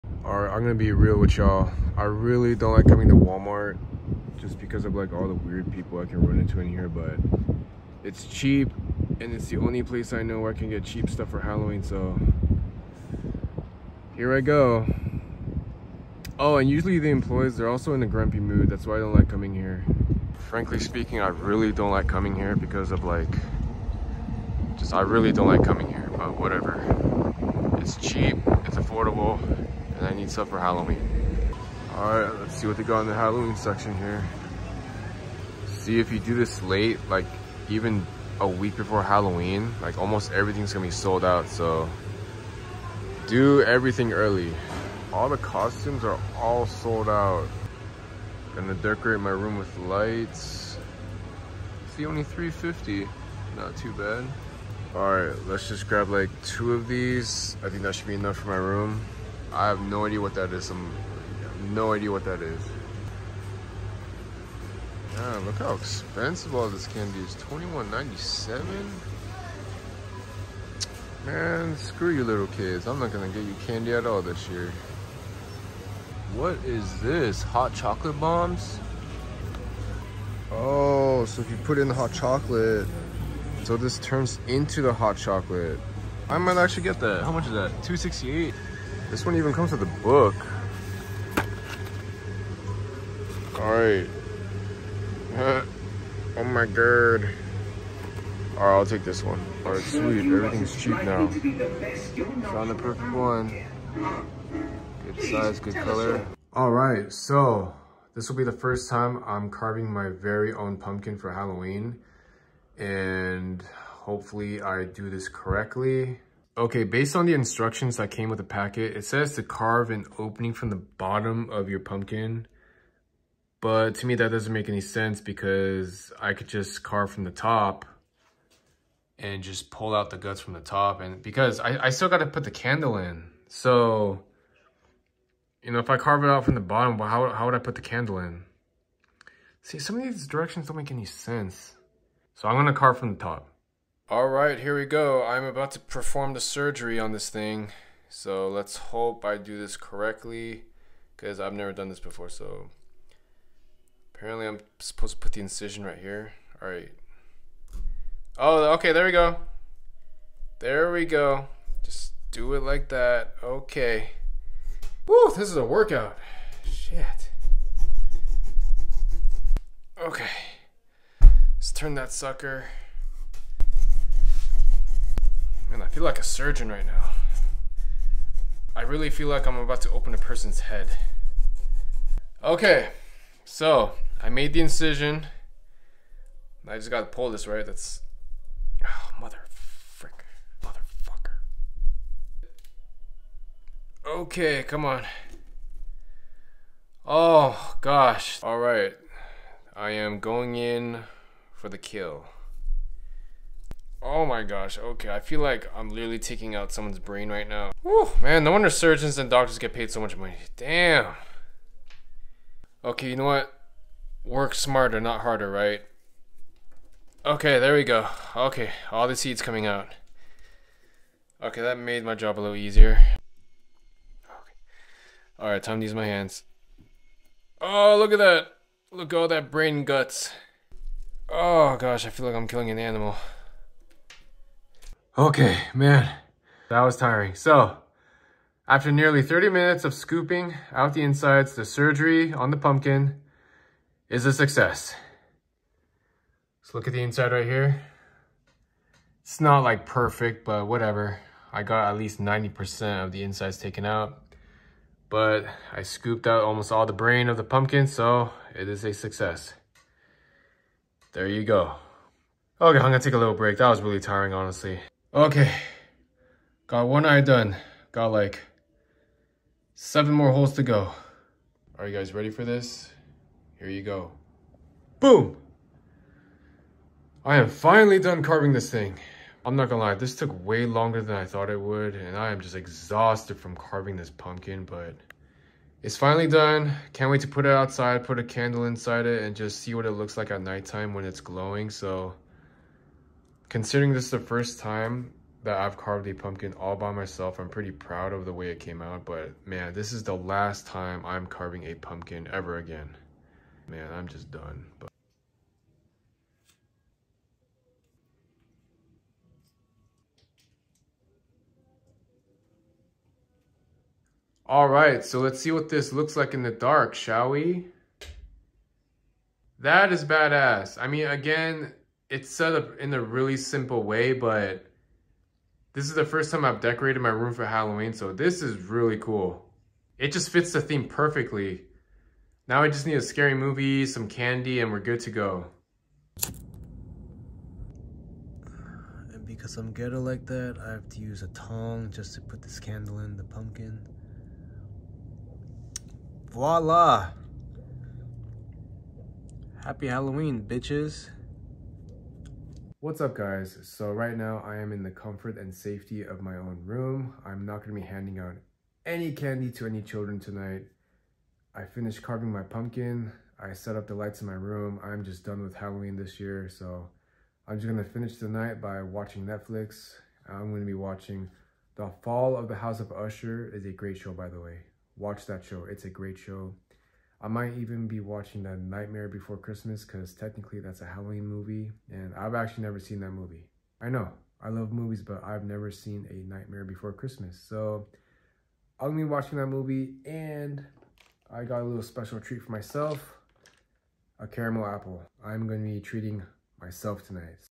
Halloween vlog: carving my first pumpkin and the lazy way of decorating my room
This year I wanted to celebrate Halloween more so I decided to put lights in my room and actually carve my own pumpkin. Enjoy.